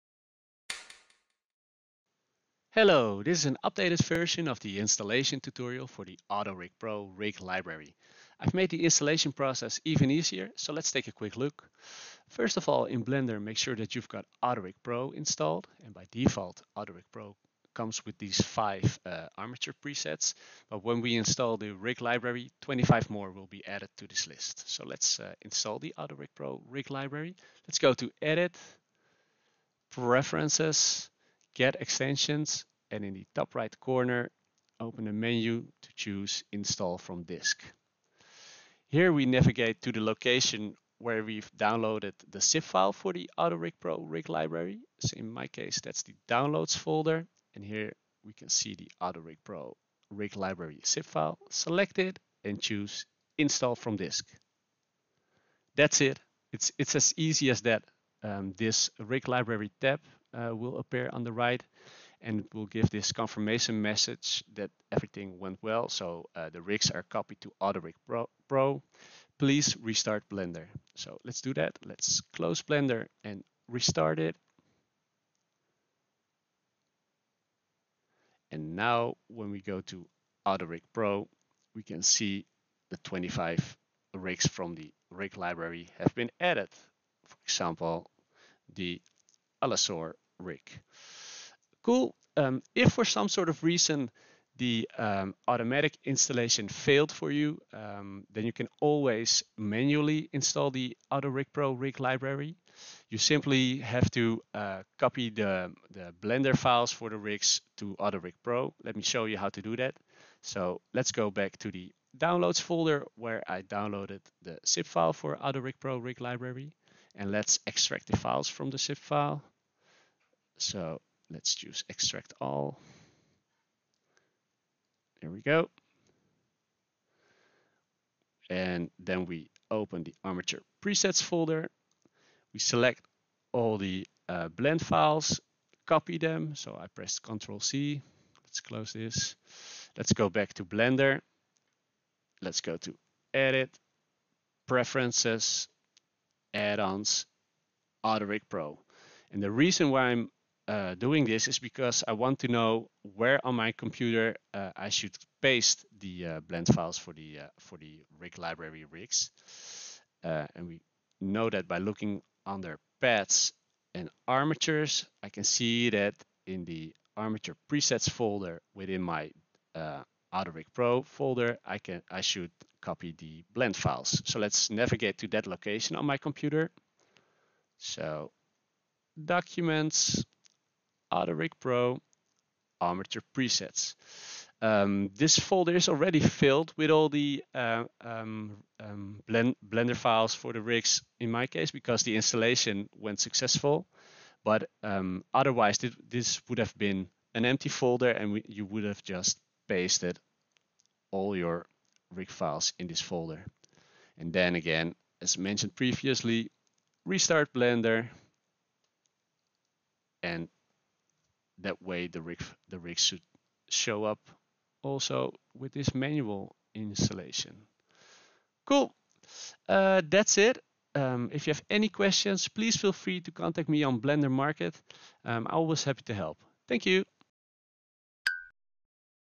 Auto-Rig Pro: Rig Library - Installation Tutorial
A Blender rig library of 25 Auto-Rig Pro animal rigs, available on Superhive.

This video demonstrates how to install the Auto-Rig Pro: Rig Library.

IMPORTANT: This rig library requires Auto-Rig Pro.

0:00 Automatic Installation
1:32 Manual Installation